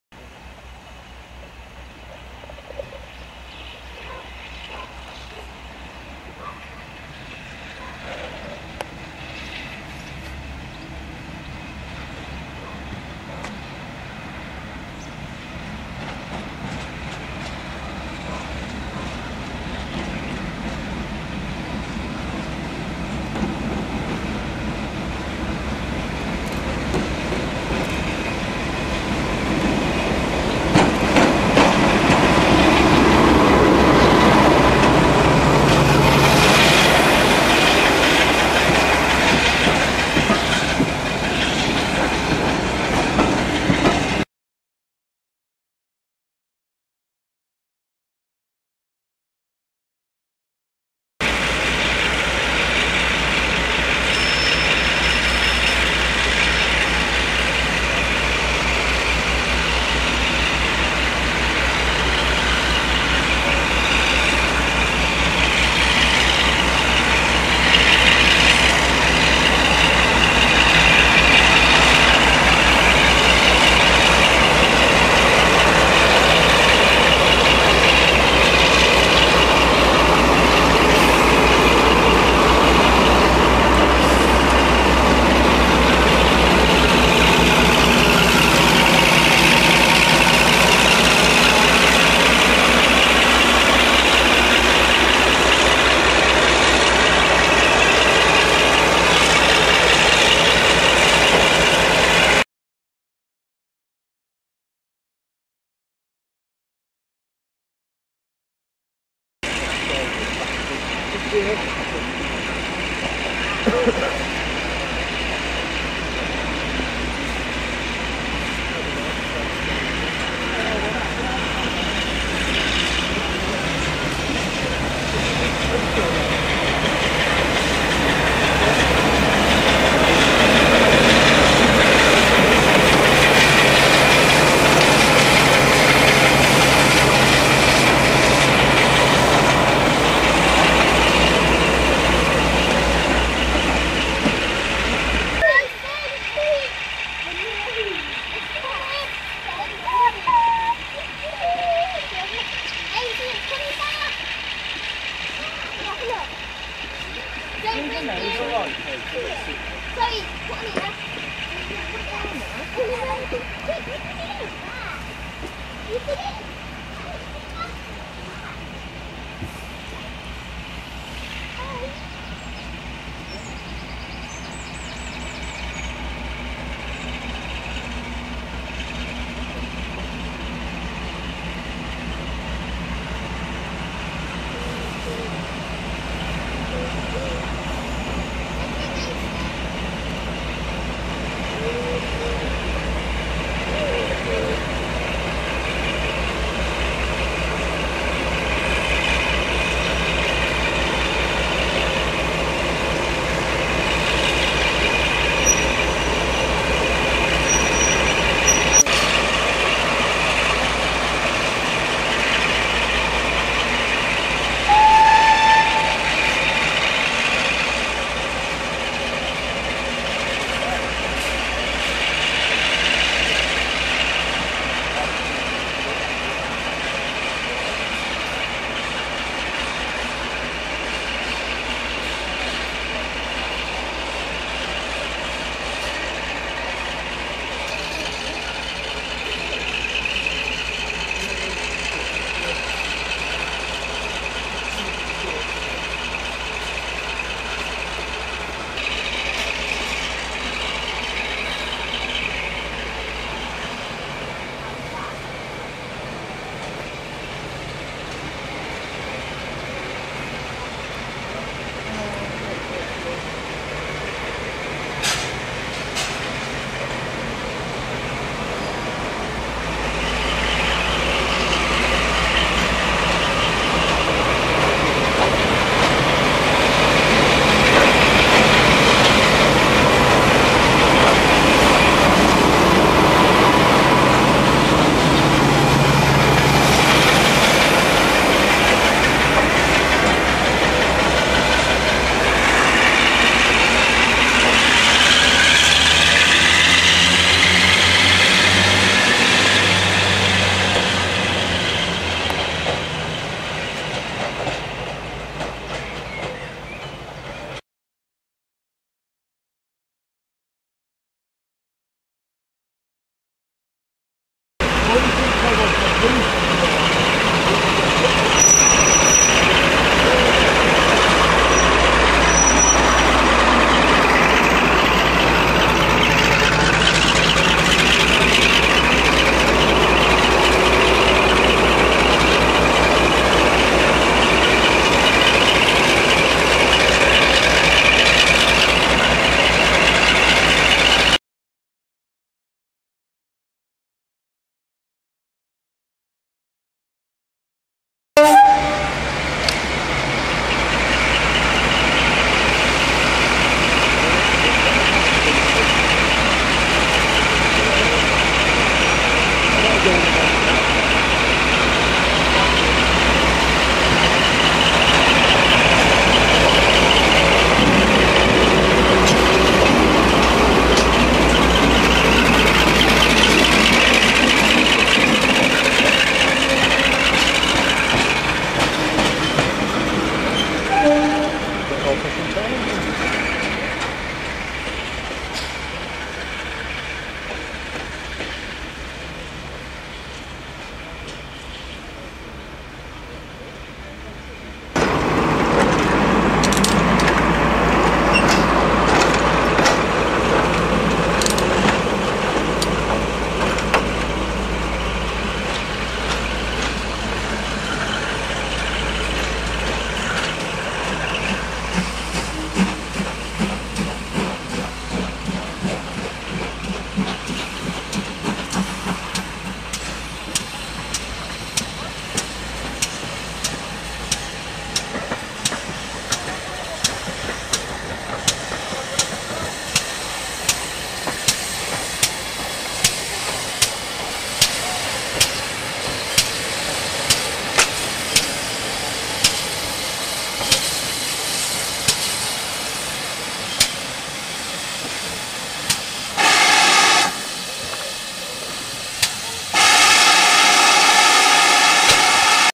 D1062 Western Courier on the SVR 13/06/2015
D1062 Western Courier worked two round trips on the 13th of June 2015. Here is some footage of D1062 at Kidderminster and Bridgnorth 13/06/2015